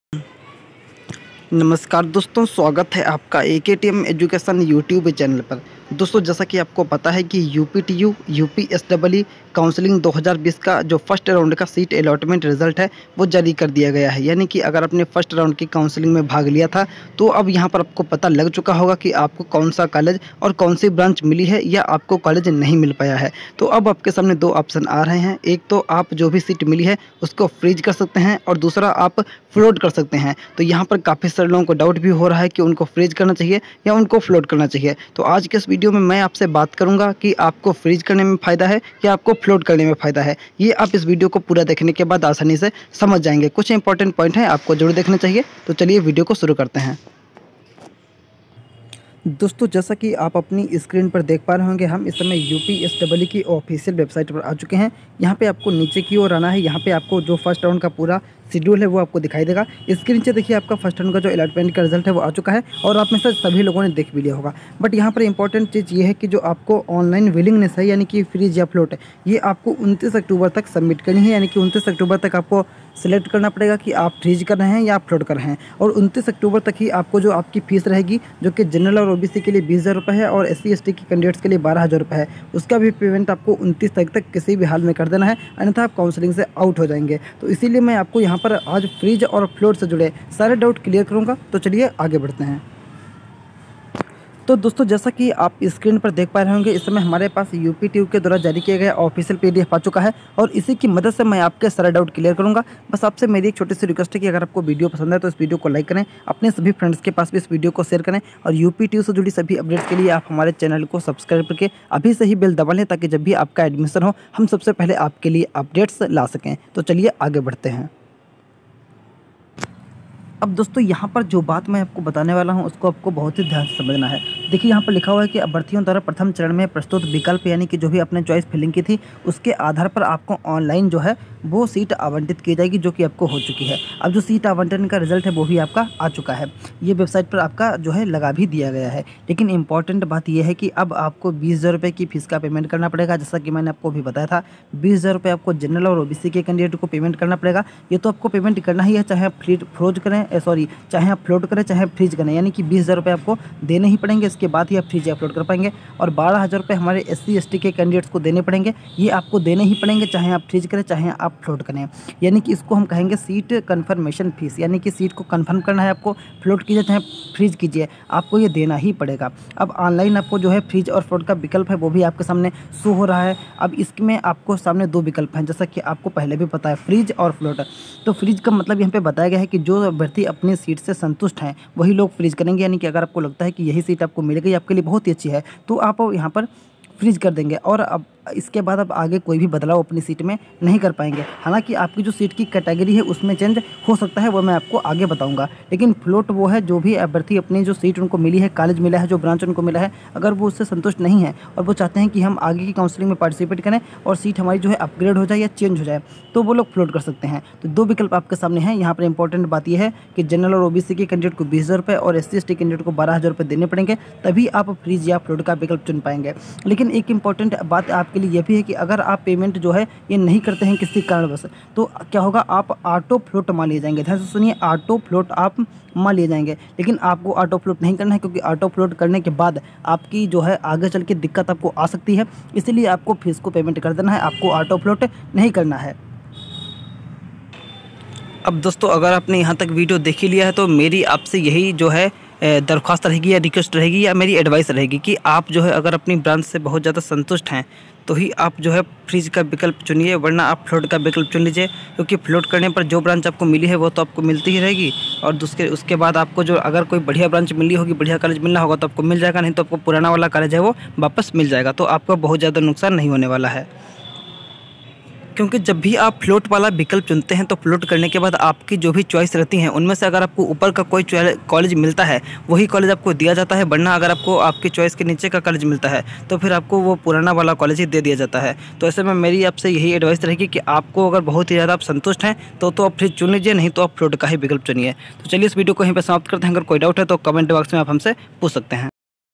Freeze or Float || UPTU/UPSEE Freeze or Float || UPTU Counselling 2020
UPTU/UPSEE Freeze or Float फायदा किसमें है 🤔 UPTU Counselling 2020 || UPSEE Counselling 2020

🙏 नमस्कार दोस्तों 🔥AKTM Education🔥
👉 YouTube चैनल पर आपका स्वागत है|

🙏 Video को देखने के लिए आपका धन्यवाद|